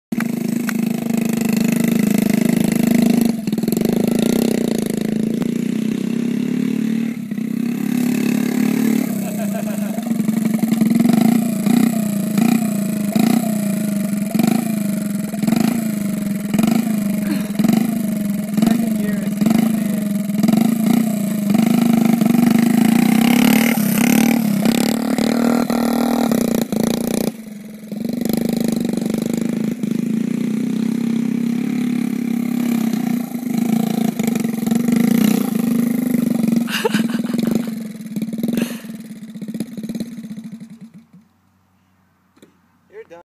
Chris' birthday: Radio Flyin'
Motorized Radio Flyer little red wagon for Chris' dirty 30 birthday? Why not? I love my friends.